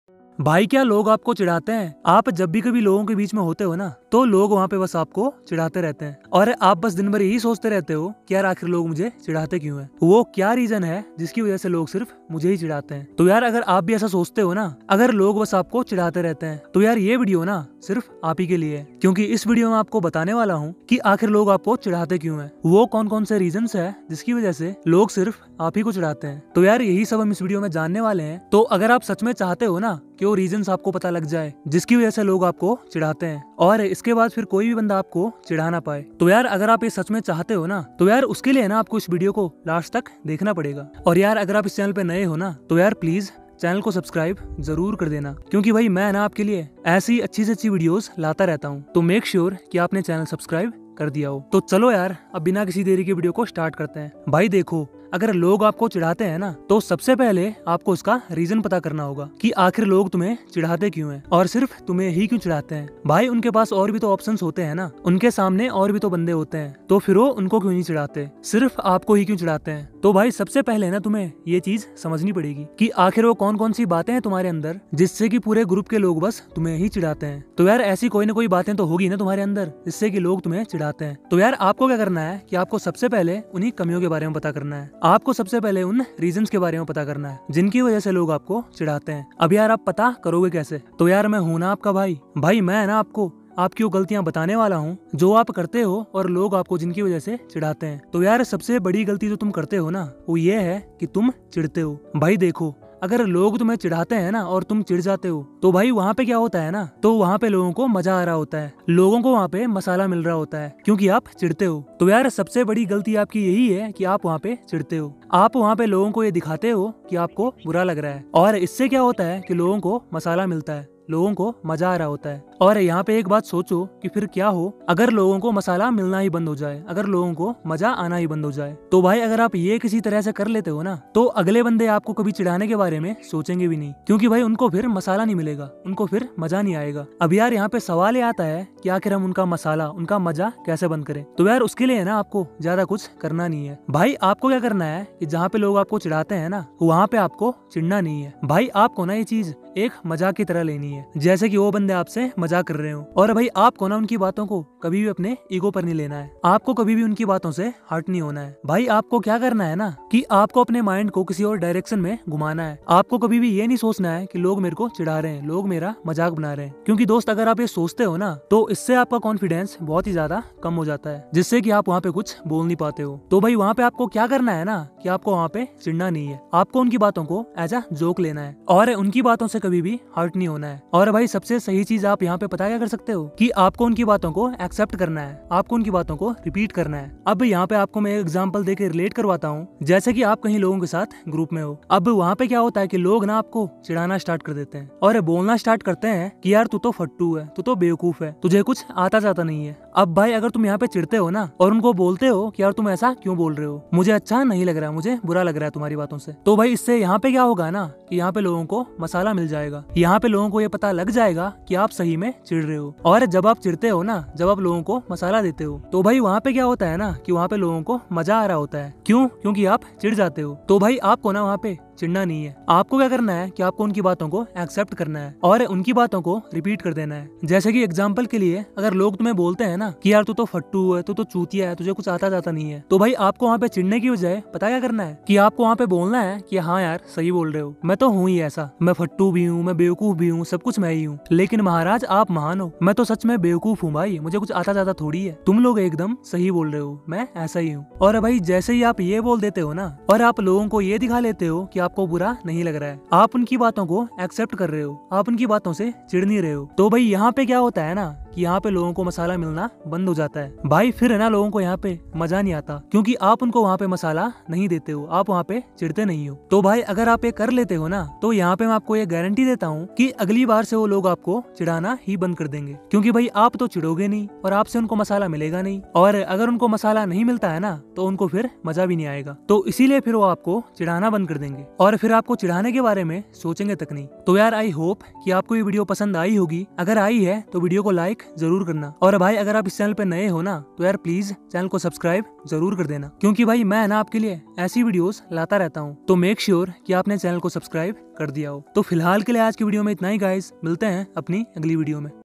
Kya Log Aapka Majak udate hai | Log mera majak udate hai kya karu | Log Chidhate hai kya kare
Is Video me maine aapko bataya hai ki Agar log aapko chidhate hai to aapko kya karna chahiye,mere pass bahut sare question aate hai ki sir, Log mera Majak udhate hai , Log mera majak banate hai kya karu, Log mujhe bahut Chidhate hai kya karu, is sawal ka jawab aapko is video me mil jayega.